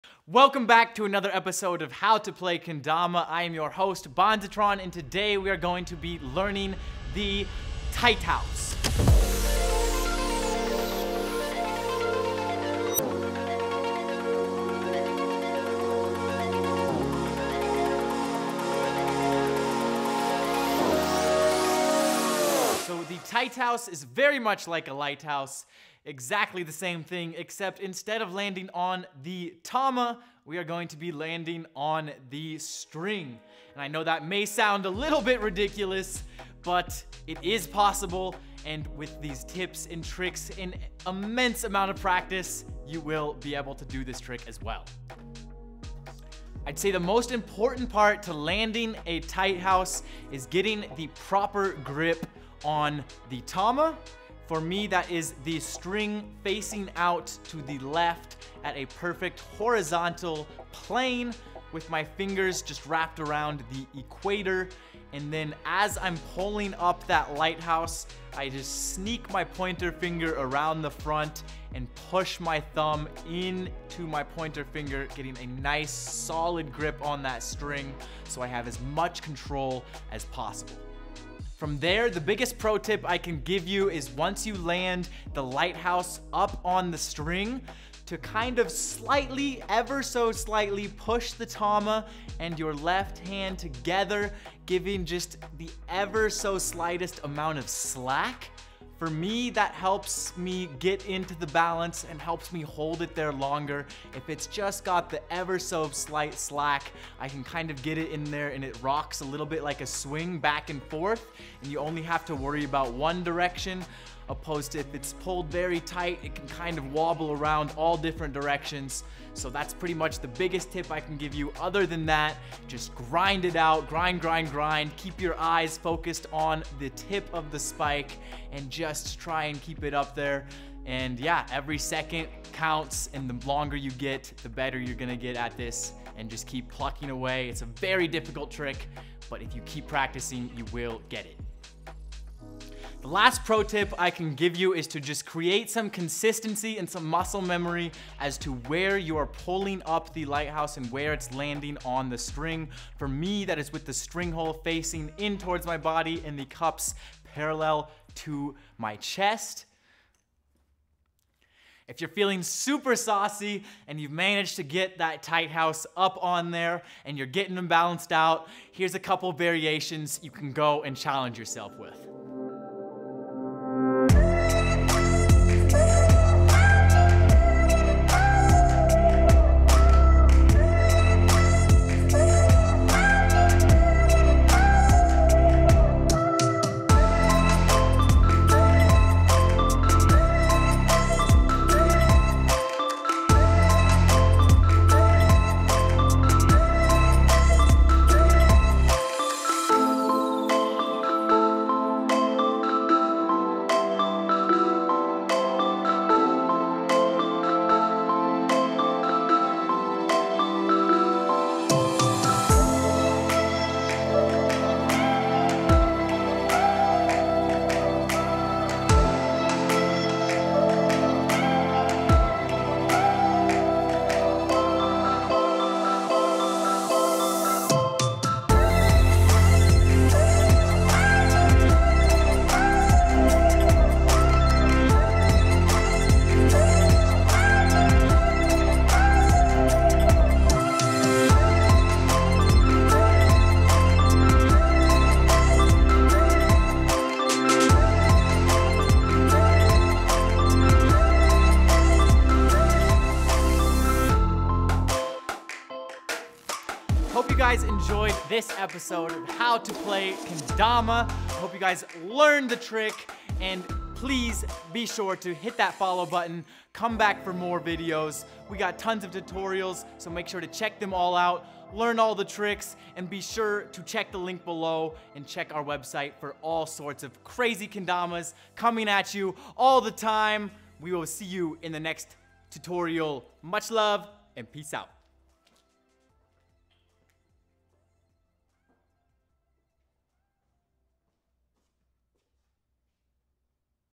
KROM "HOW TO PLAY KENDAMA" Tight House
DON'T MISS OUT! Scoop up a fresh kendama at KROMKENDAMA.com!

In this quickie tutorial, you'll learn from kendamas VERY best BONZ ATRON how to tight house.

Don't stress it. Chill out, focus, refocus, and stomp the trick! You got it.

- krom

⭕ BE A PART OF THE KROM ONLINE PARTY! ⭕